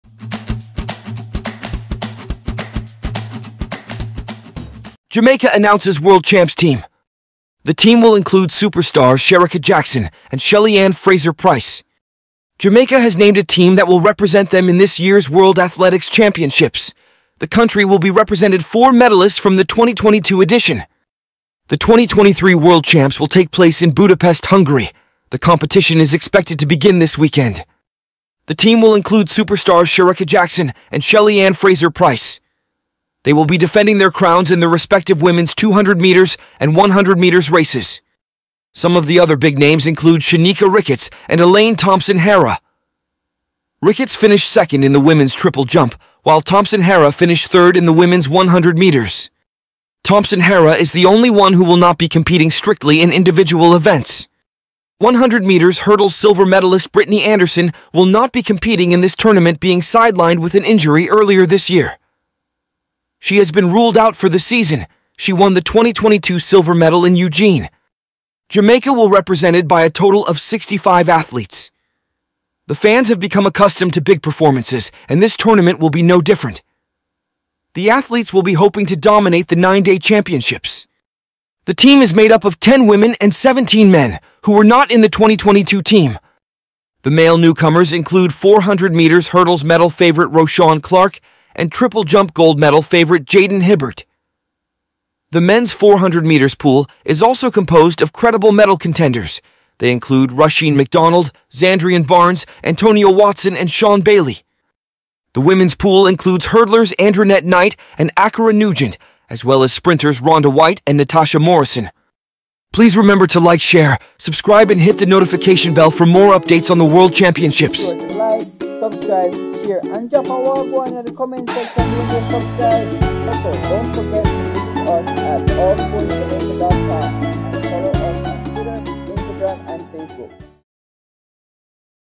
Jamaica's Star Team for World Athletics Championships 2023 Shericka Jackson, Shelly-Ann Fraser-Pryce
🏃‍♀️ Get ready for an electrifying showdown on the track! 🌟 In this episode, All Sports Jamaica dives into the exciting lineup of Jamaica's team for the 2023 World Athletics Championships in Budapest, Hungary. 🇯🇲

With the competition kicking off this weekend, Jamaica is set to make its mark with a squad of 65 exceptional athletes. The team features a combination of seasoned champions and promising newcomers, all hungry for victory.

🥇 Defending their crowns are the dynamic duo of Shericka Jackson and Shelly-Ann Fraser-Pryce, who will compete in the women’s 200m and 100m races, respectively. But they're not alone! 🚀 Joining them are stars like Shanieka Ricketts and Elaine Thompson-Herah, each aiming to continue their remarkable performances.

However, the absence of 100m hurdles silver medalist Britany Anderson, sidelined due to injury, adds a twist to the competition. But fear not, as Jamaica's talent pool is deep and diverse, showcasing talents like 400m hurdles medal favorite Roshawn Clarke, triple jump gold medal contender Jaydon Hibbert, and a strong men's 400m contingent including Rusheen McDonald, Zandrion Barnes, Antonio Watson, and Sean Bailey.

The women's side is equally impressive, featuring hurdlers Andrenette Knight and Ackera Nugent, along with sprinters Ronda Whyte and Natasha Morrison, all prepared to seize victory.

📅 With nine days of thrilling action ahead, Jamaica's athletes are geared up to dominate the World Athletics Championships and leave a legacy of excellence on the global stage. 🌍

📷 Connect on Instagram: @AllSportsJamaica
📱 Join the conversation on Facebook: AllSportsJamaica

I do not intend to claim the copyright of any game video uploaded.